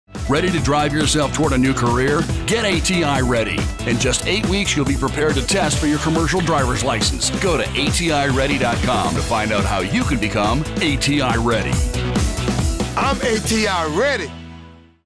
ATI CDL Licensed
Ready to drive yourself towards a new career? In just 8 weeks you'll be prepared to test for your commercial drivers license.

For information on program success, costs, and other important facts, Certified to operate by SCHEV.